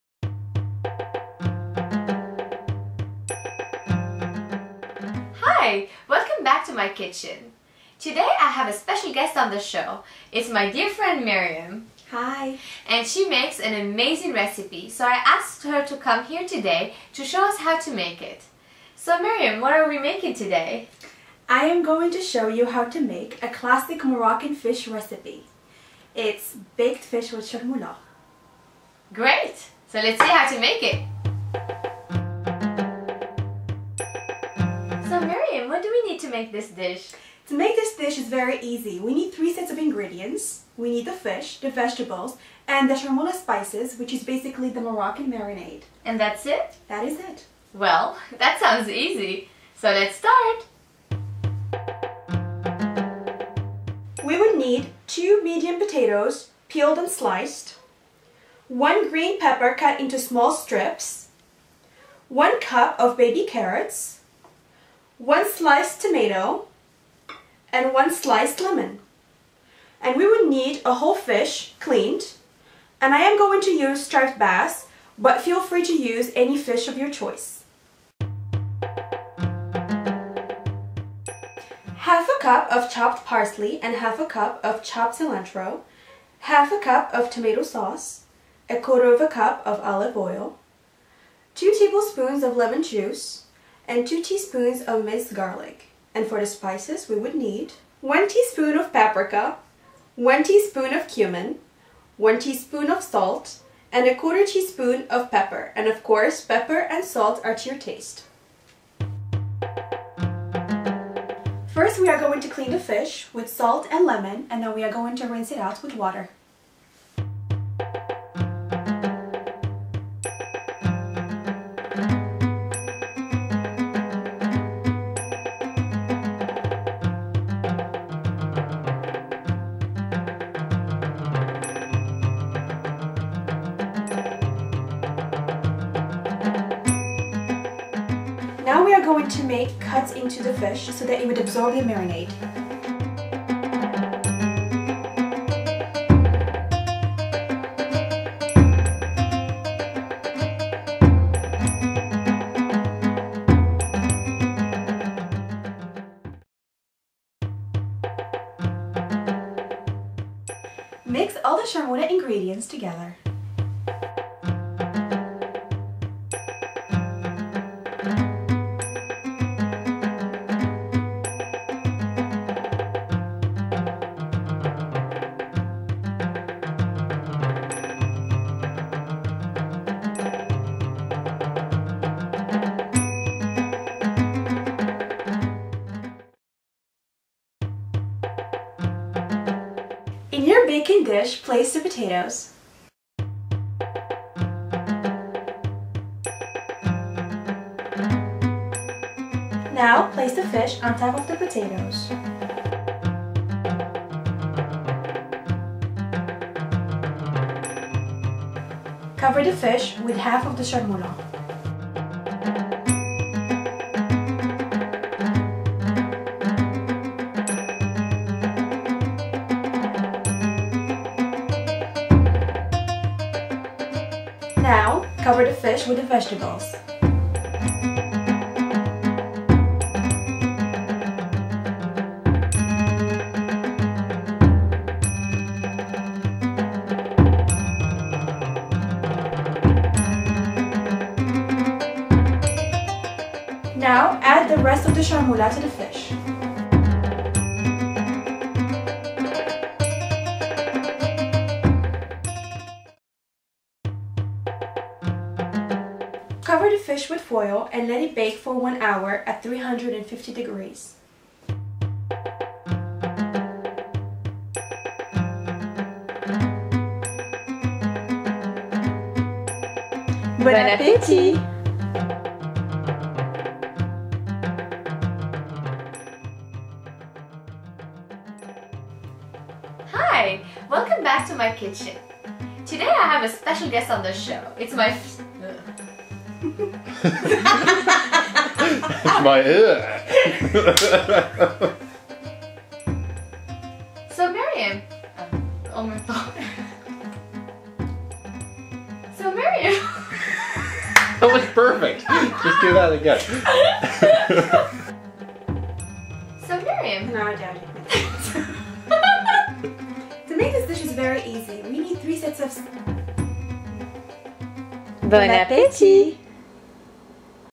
Moroccan Baked Fish Recipe - CookingWithAlia - Episode 55
How to make a classic Moroccan recipe: Baked fish with Charmoula, a delicious Moroccan marinade.

* If you'd like to watch a Dada (traditional Moroccan cook) make this dish, check out this video